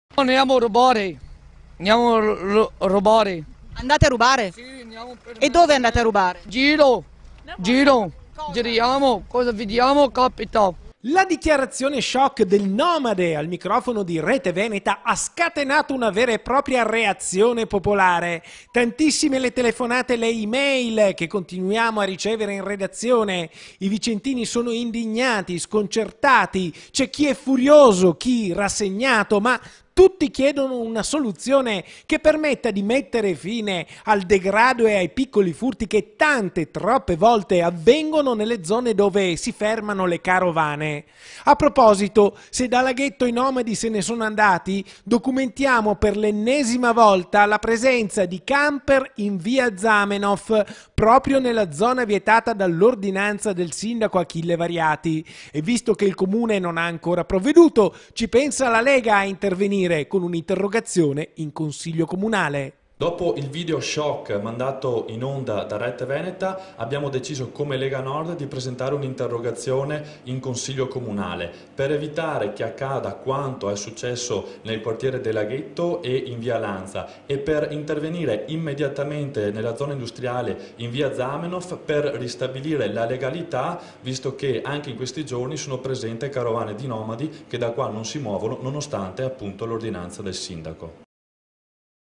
TG VICENZA (22/08/2017) - INTERVISTA CHOC, L'INTERROGAZIONE DELLA LEGA
TG VICENZA (martedì 22 agosto 2017) - La dichiarazione choc del nomade al microfono di Rete Veneta ha scatenato una vera e propria reazione popolare: tantissime le telefonate e le e-mail che continuiamo a ricevere in redazione: i vicentini sono sconcertati, c'è chi è furioso, chi è rassegnato, ma tutti chiedono una soluzione che permetta di mettere fine al degrado e ai piccoli furti che tante, troppe volte avvengono nelle zone dove si fermano le carovane. A proposito: se da Laghetto i nomadi se ne sono andati, documentiamo per l'ennesima volta la presenza di camper in via Zamenhof, proprio nella zona vietata dall'ordinanza del Sindaco Achille Variati. E visto che il comune non ha ancora provveduto, ci pensa la Lega a intervenire, con un'interrogazione in Consiglio Comunale.